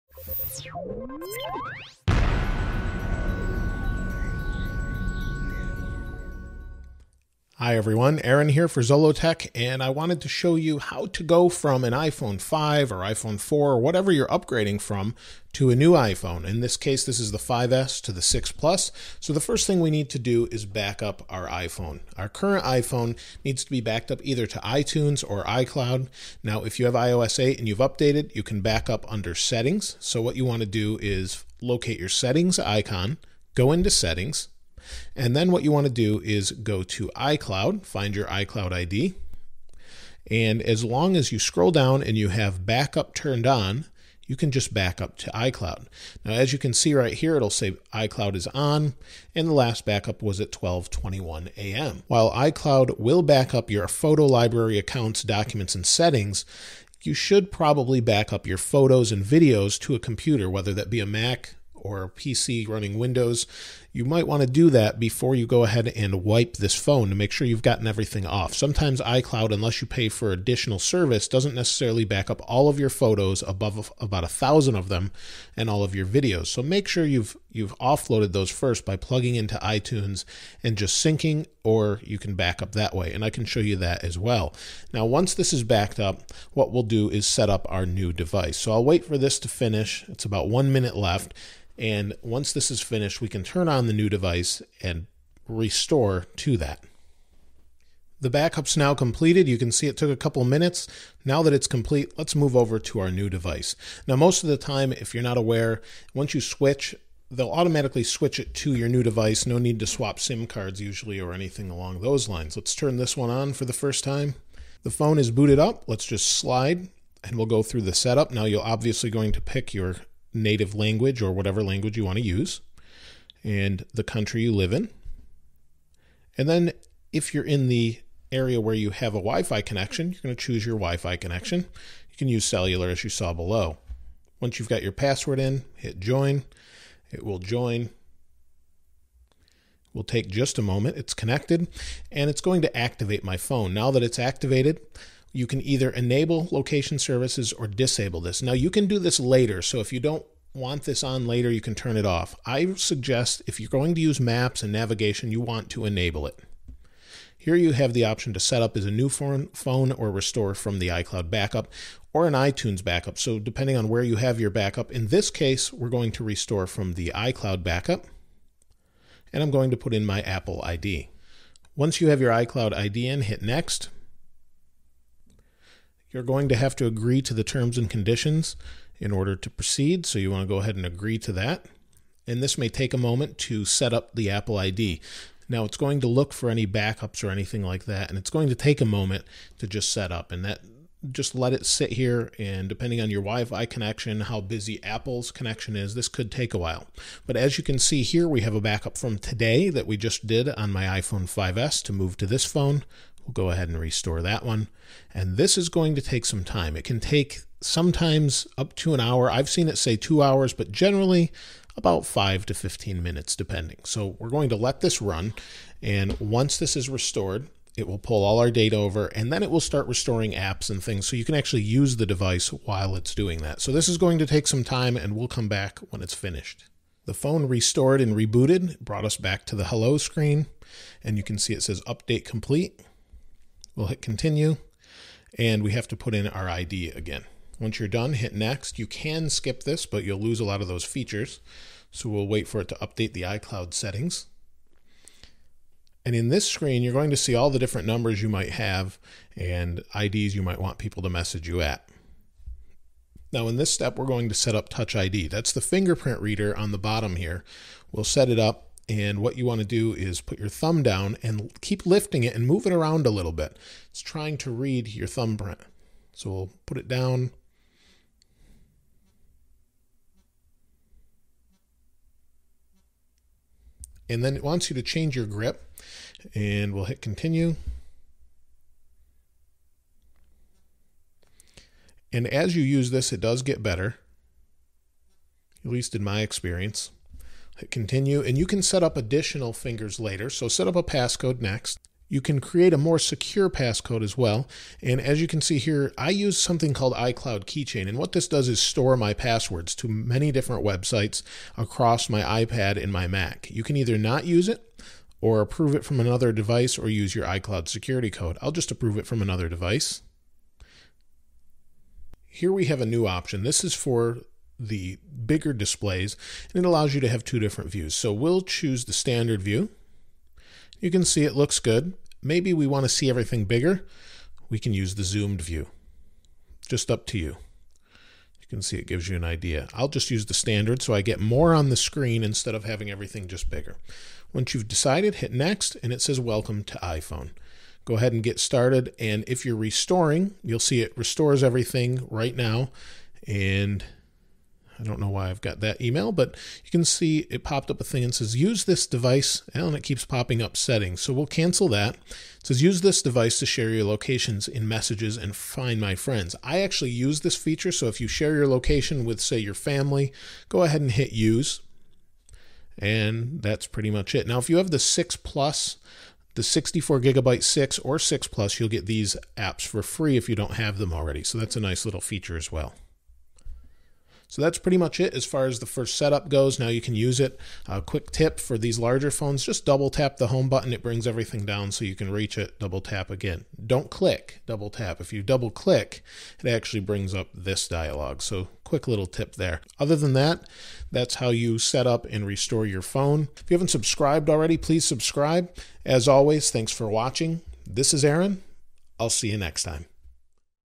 How to Backup Your Old iPhone and Restore to iPhone 6
If you're upgrading to the iPhone 6, iPhone 6 Plus or even from a 4S to an iPhone 5, this guide will help you back up your information and get it ready to restore on your new device.  I walk you through using iCloud backup and show you how it all works.
iCareFone - One click to backup and restore iPhone from backups files, fix iPhone stuck during restore.